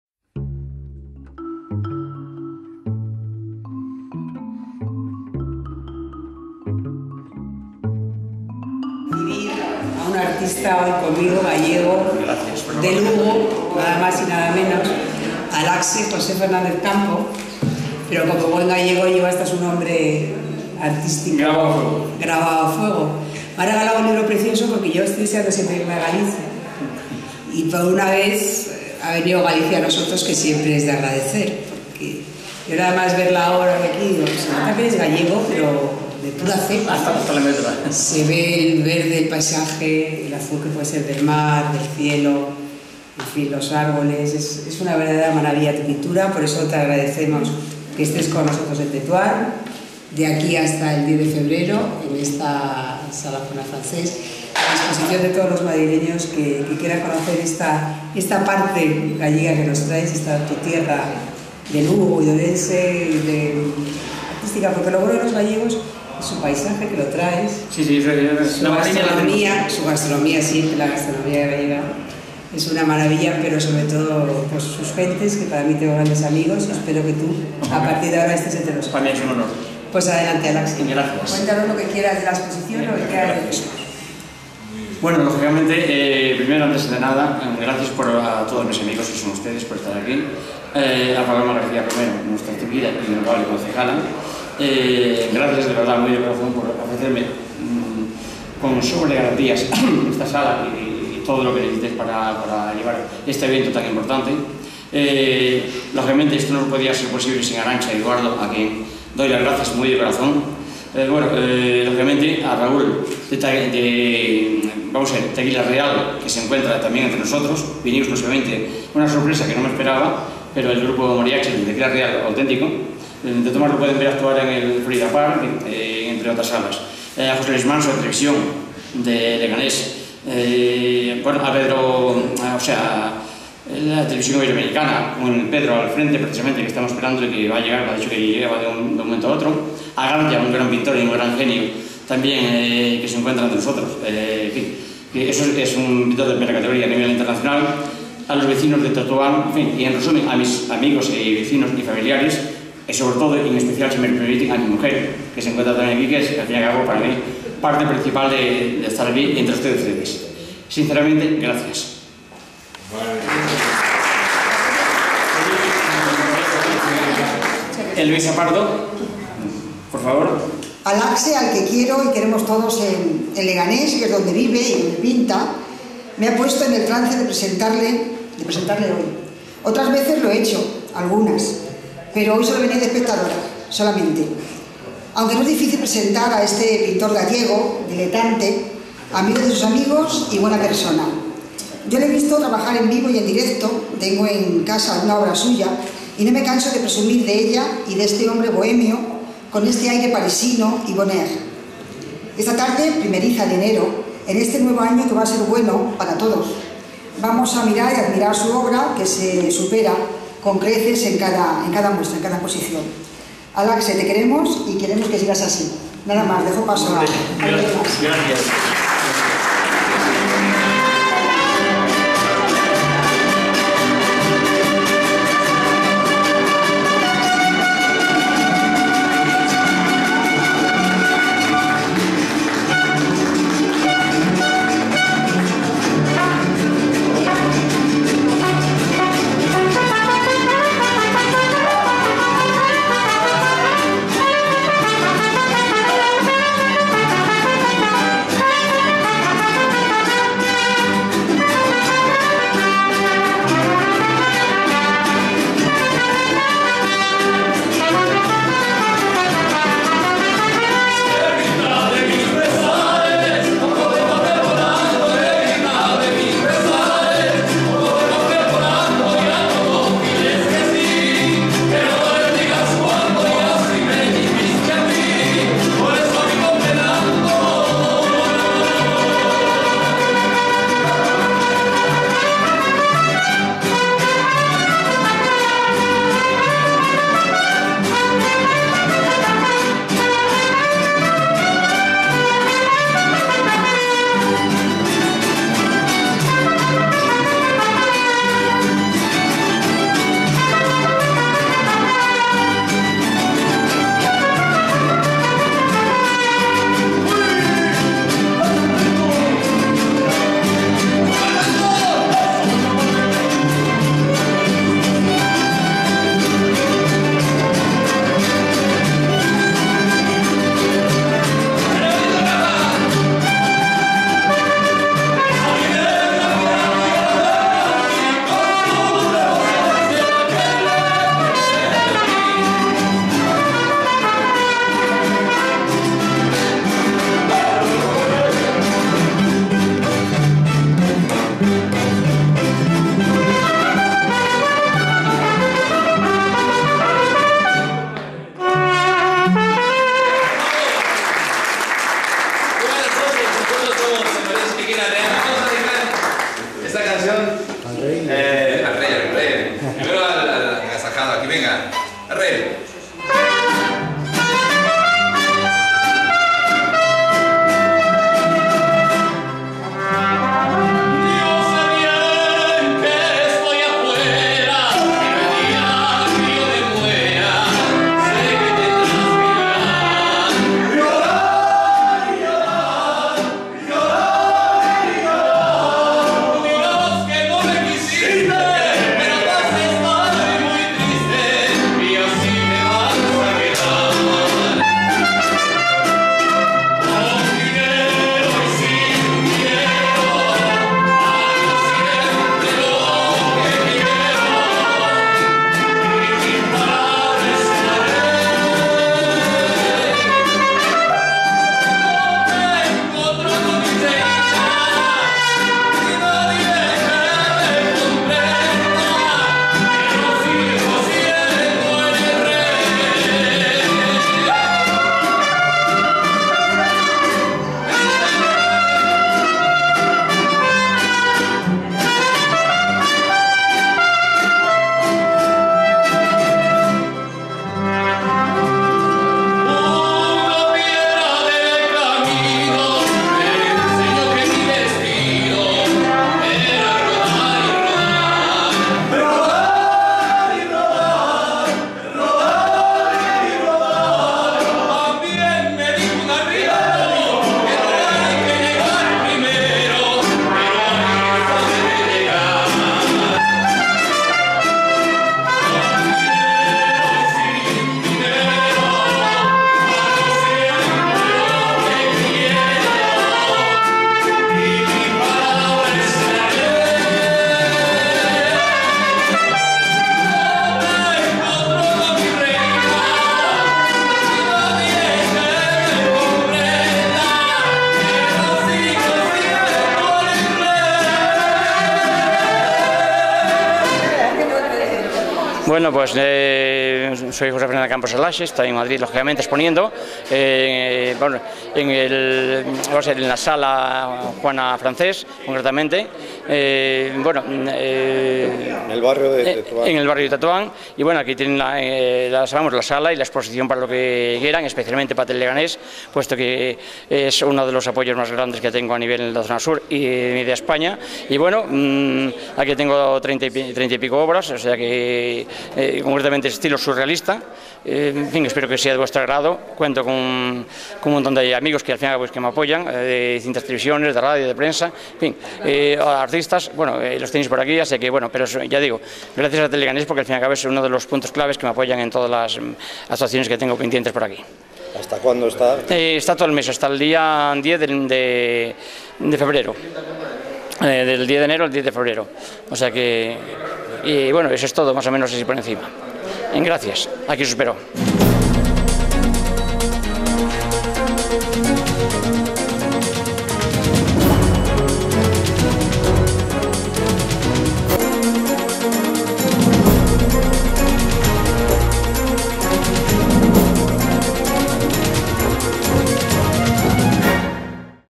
EXPOSICIÓN PICTORICA DE 'ALAXE' EN MADRID
El pasado martes 10 de enero, se inauguró una exposición del artista gallego José Fernández Campos "ALAXE" en la sala de exposiciones Juana Francés del distrito madrileño de Tetuán, que permanecerá abierta al público hasta el 10 de febrero de 2012.
Teleganés grabó los mejores momentos de la citada inauguración.